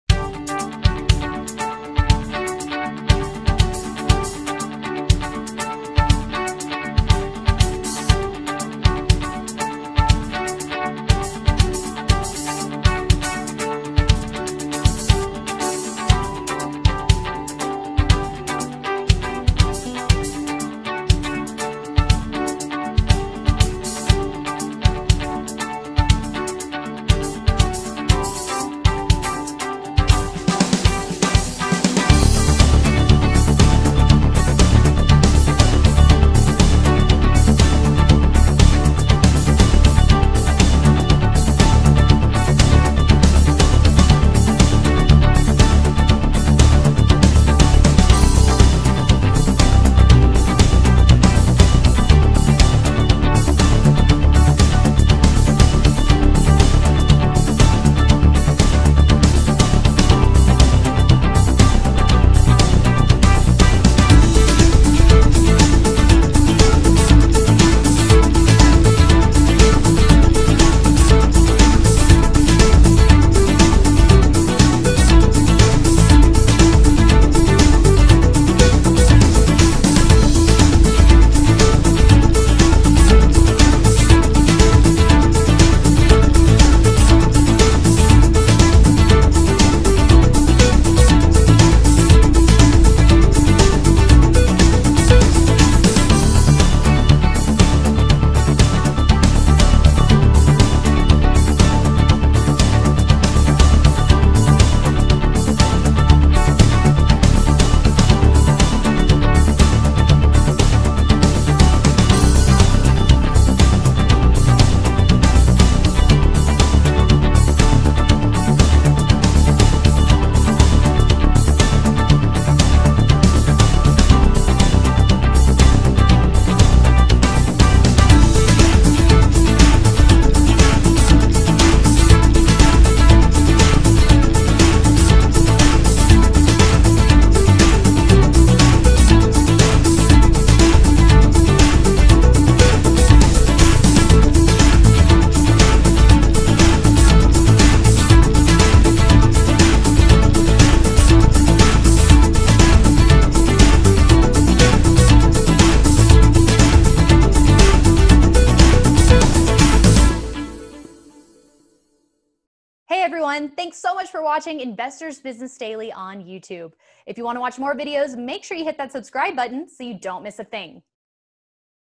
2020 Election Results: Biden's Win Should Boost These IRA, 401(k) Investments
Biden took the 2020 election, and it is reported that this should boost these IRA and 401(k) investments.